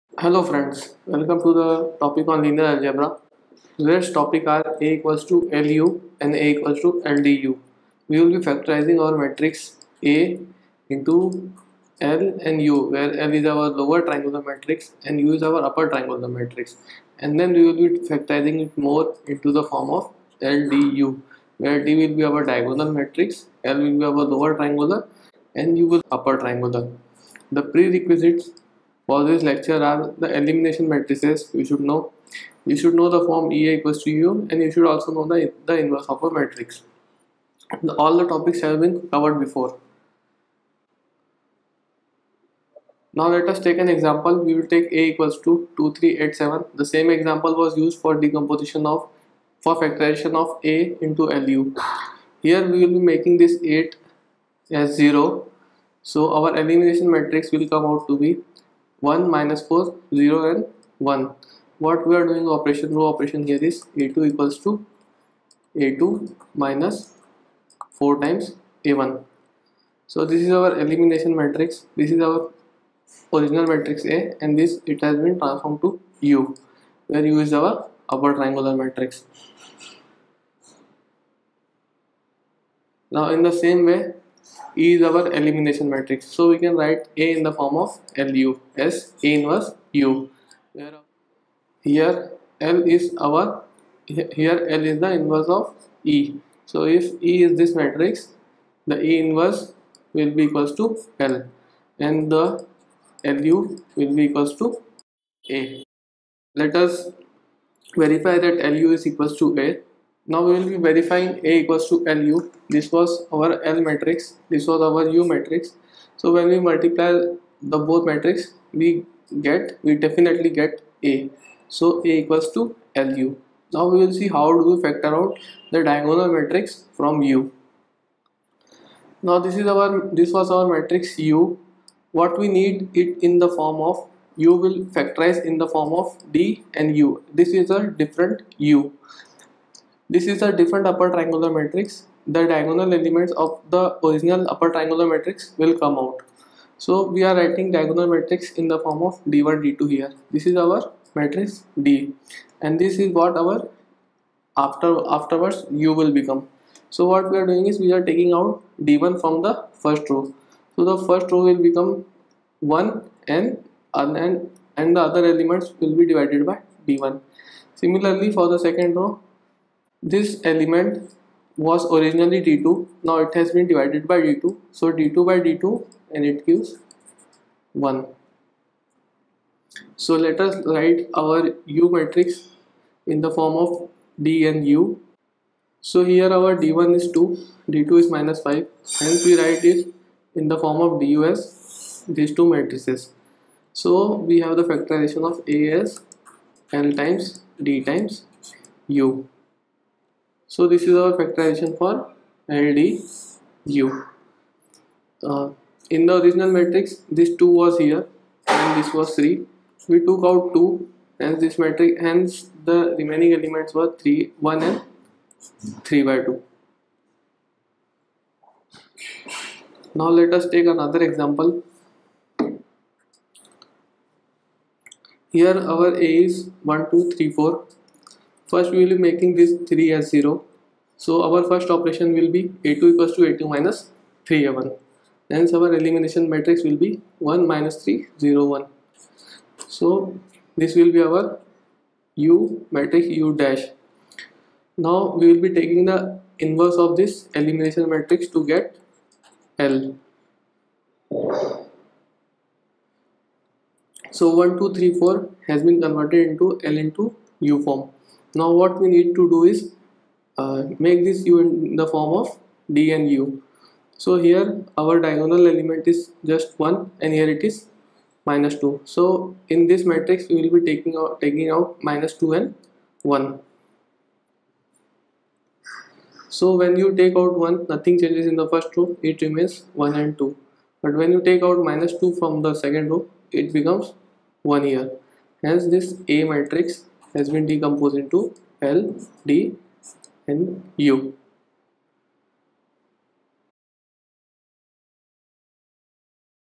Linear Algebra - 7 | A = LU | A = LDU | Matrix Factorization | Blue Board Academy
Linear Algebra Lecture 7.
Linear Algebra Crash Course
This video tells about the Matrix Factorization A = LU and A = LDU

The content presented is by Blue Board Academy.